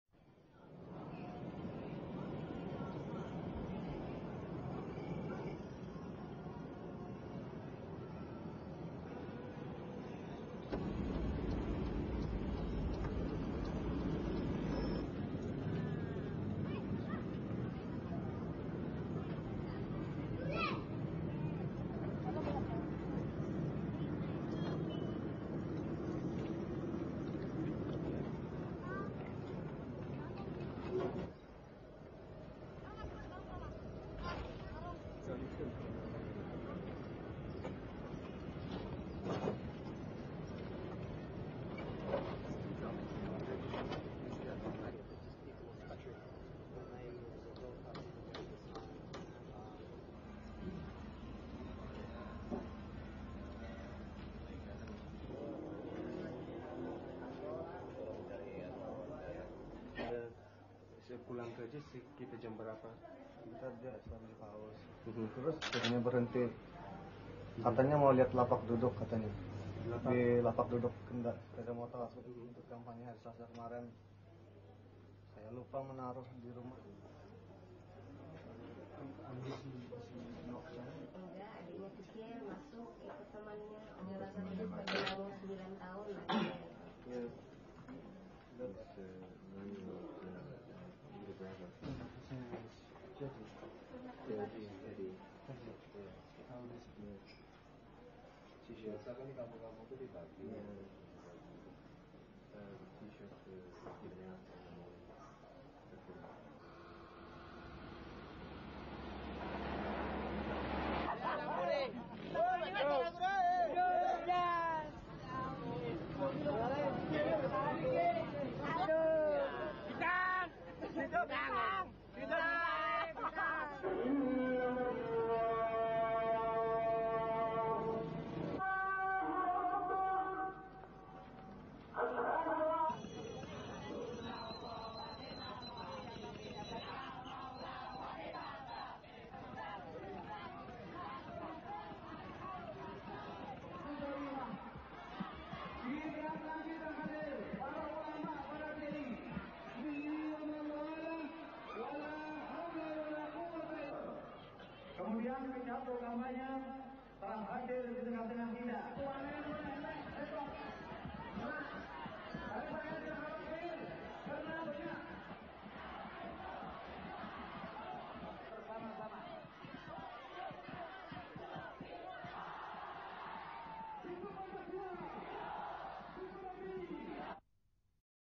Kerusuhan Kampanye Pemilu 1997 (Amuk Massa Banjarmasin 23 Mei 1997)
KERUSUHAN BANJARMASIN 23 MEI 1997